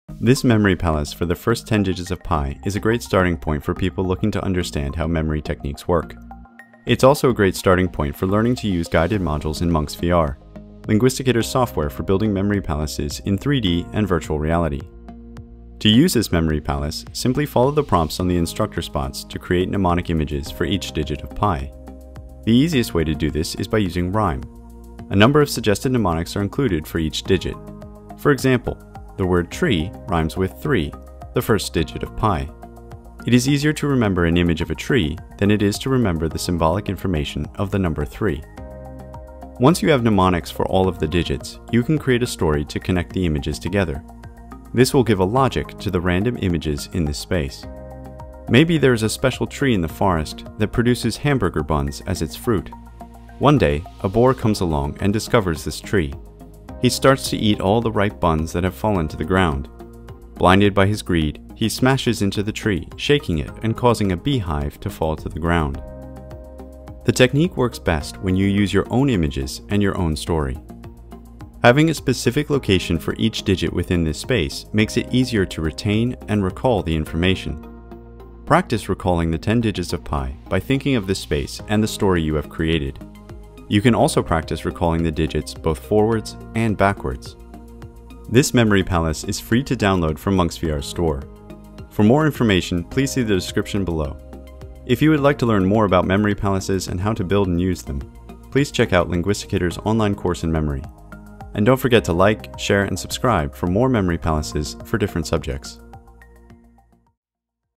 How to Remember Pi with a Memory Palace in Munx VR
This free memory palace in Munx VR is a great starting point for learning about memory techniques in general, and guided memory palaces in Munx VR specifically. Explore the space, create mnemonics for each digit of pi, and create a silly story to connect the images together. You can then practice recalling the first 10 digits of pi both forwards and backwards by thinking of this space in your mind.

You can use this memory palace on a desktop or explore it in virtual reality.

Munx VR is available for free on Steam. Please also check out our online courses in memory and languages linked below.